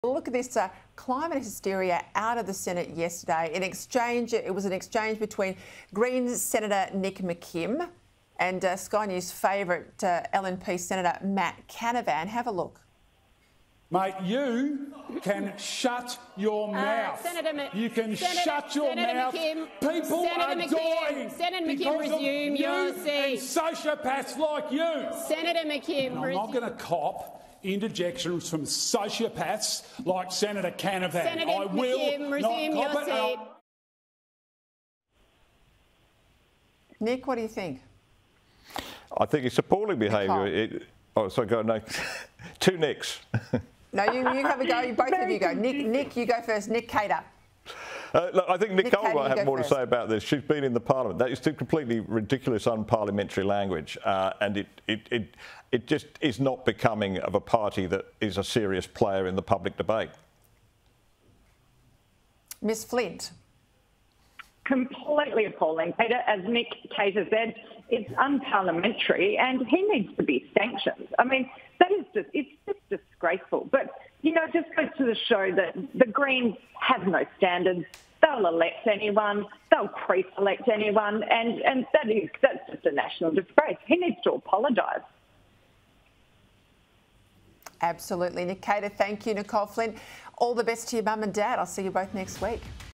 Nick McKim's unparliamentary outburst shows Greens will just elect anyone: Flint
Former Liberal MP Nicolle Flint says Greens Senator Nick McKim behaved in a completely appalling and unparliamentary manner towards Nationals Senator Matt Canavan by calling him a sociopath.

“He needs to be sanctioned, it is just disgraceful,” Ms Flint told Sky News host Peta Credlin.

“It just goes to show the Greens have no standards – they’ll elect anyone, they’ll preselect anyone.”